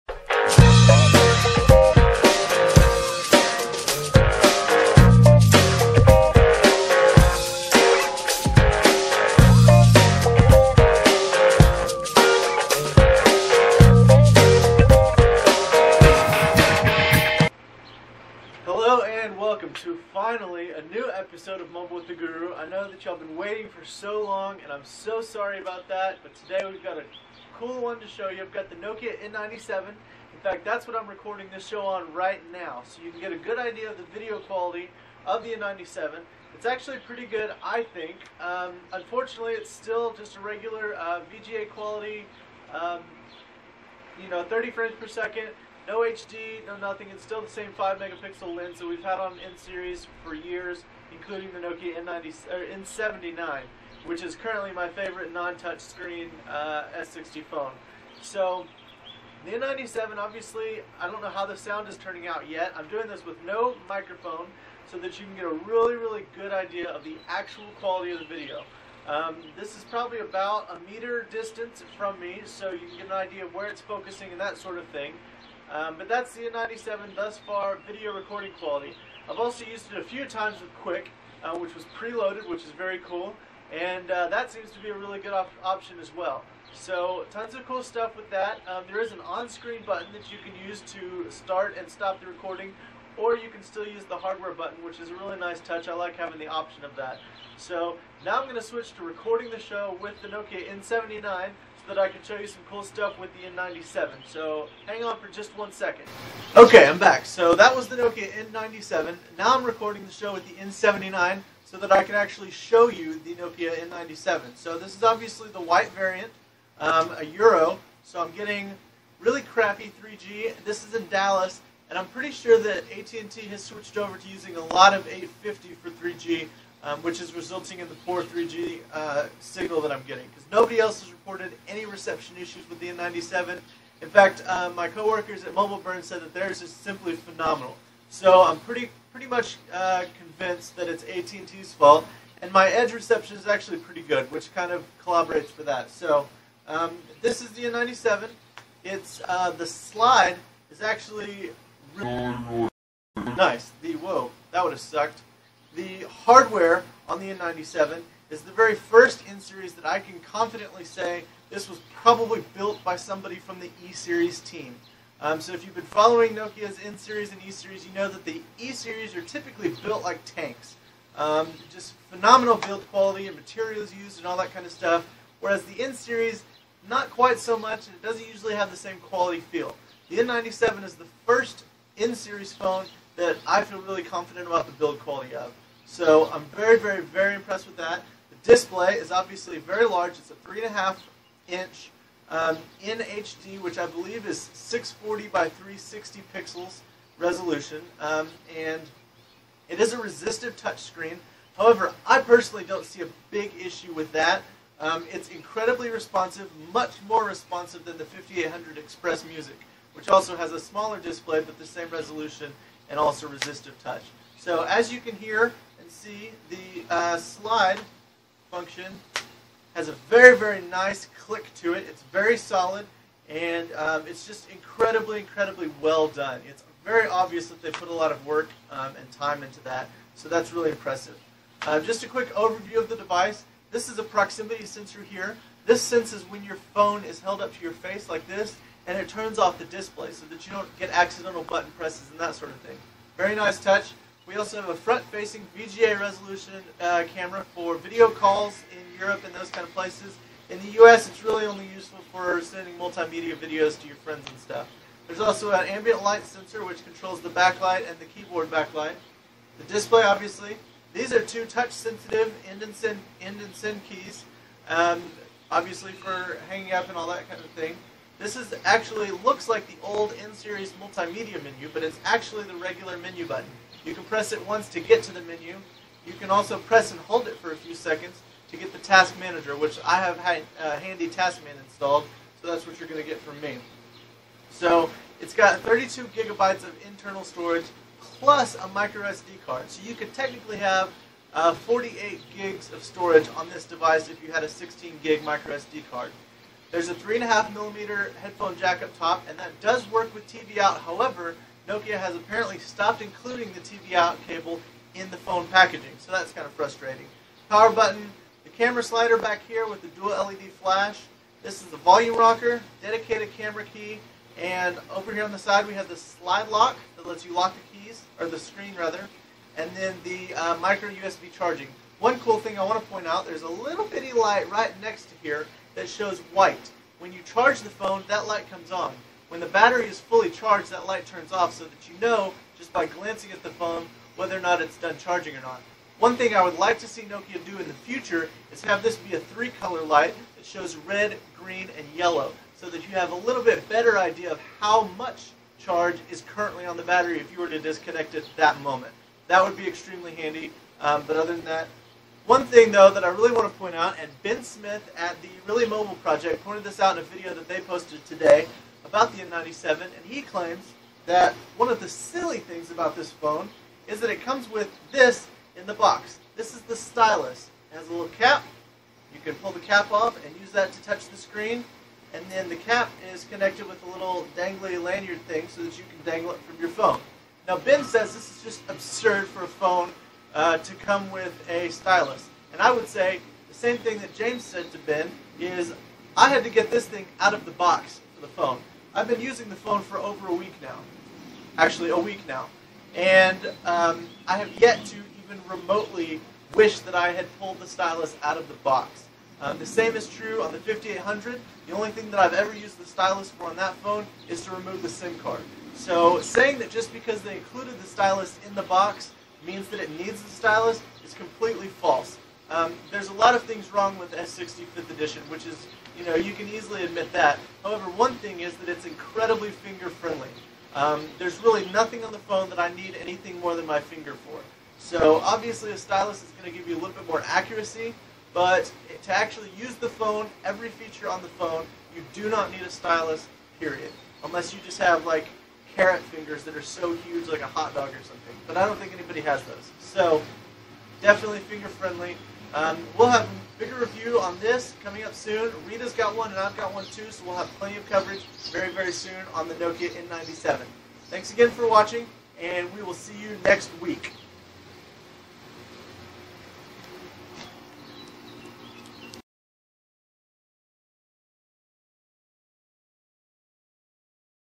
Episode 27 - N97 and N79
Episode 27 includes a look at the Nokia N97 - the first part of the video is a sample of the video recording capabilities of this touchscreen smartphone, while the second part gives an overview of the hardware, and a piece on the included stylus.  Distributed by Tubemogul.